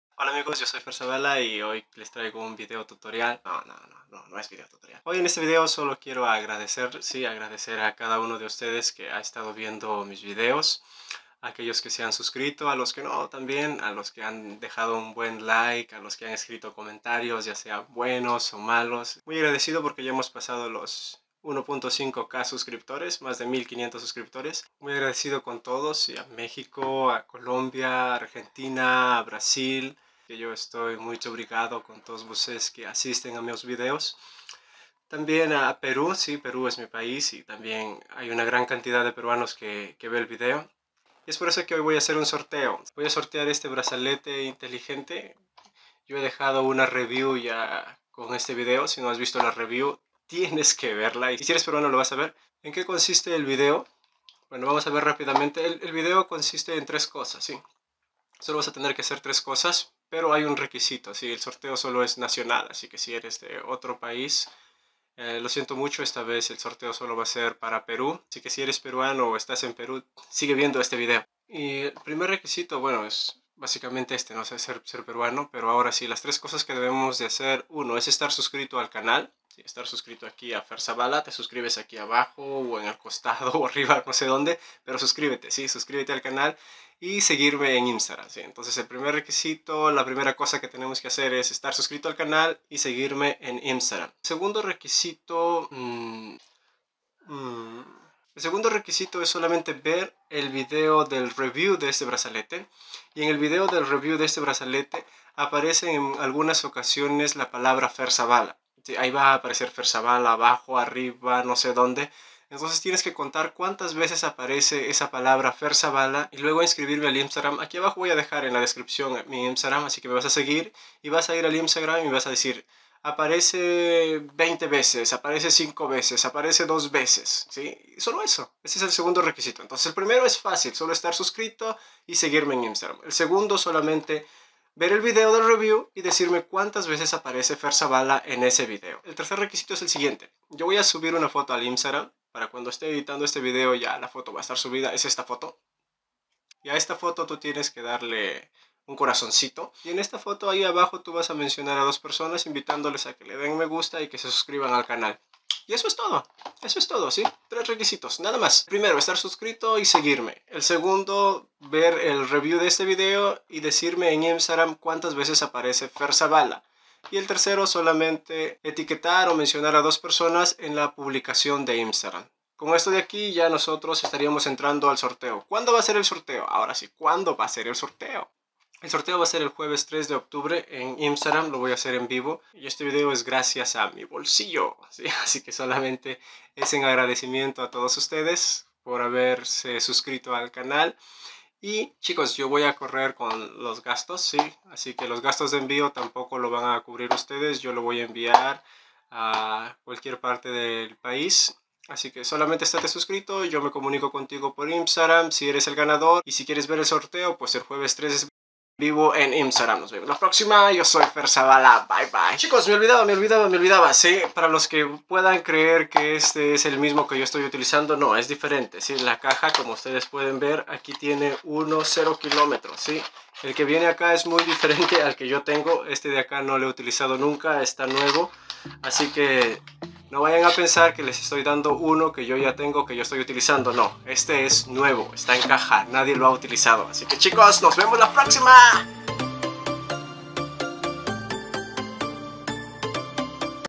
SORTEO y Agradecimiento + de 1.6 k suscriptores
llévate el Smart Band (brazalete inteligente) completamente gratis.
quieres ser parte de este sorteo, pues solo tienes que ver el vídeo.